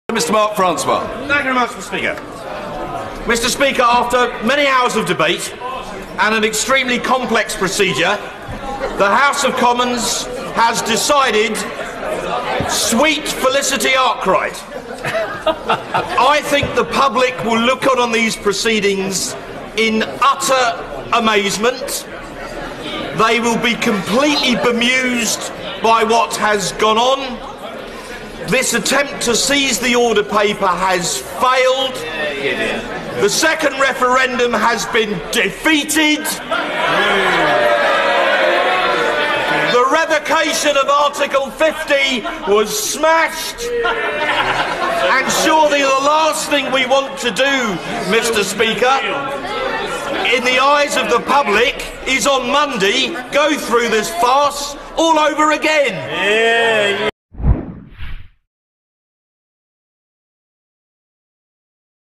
MP Mark Francois "After many hours, Commons has decided sweet F.A."
MB NEWS: MP Mark Francois "After many hours of debate the House of Commons has decided sweet F.A."

"The 2nd Referendum has been DEFEATED, the revocation of Article 50 has been SMASHED, last thing we want to do on Monday is go through this farce all over again."
BOOM 🔥🔥